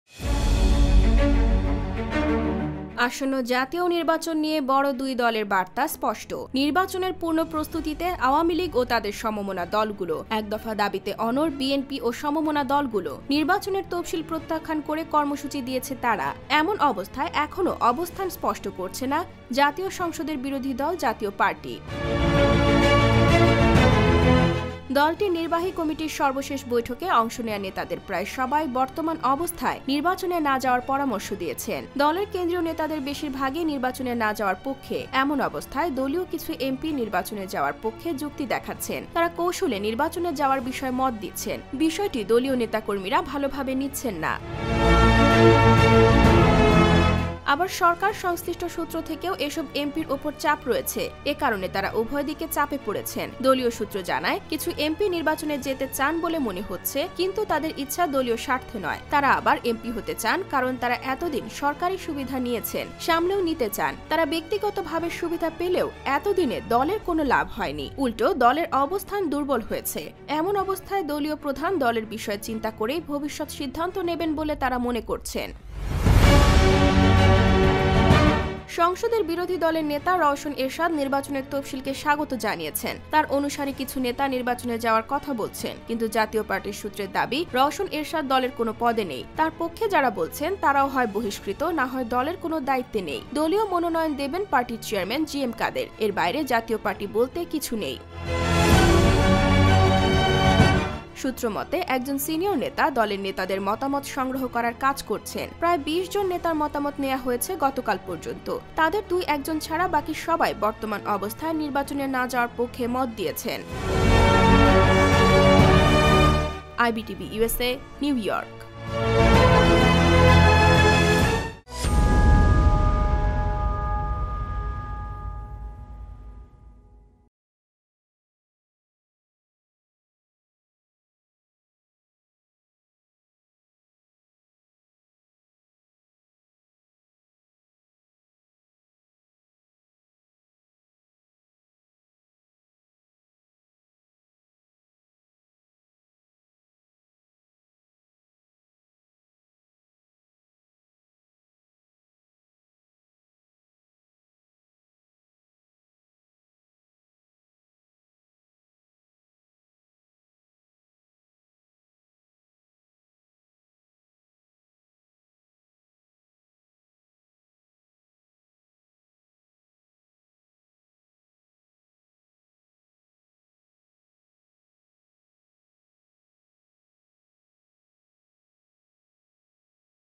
নির্বাচনে যাওয়া নিয়ে চাপে জাতীয় পার্টির এমপিরা I Jatiya Party I elections I pressure I
Jatiya Party MPs are under pressure to go to the elections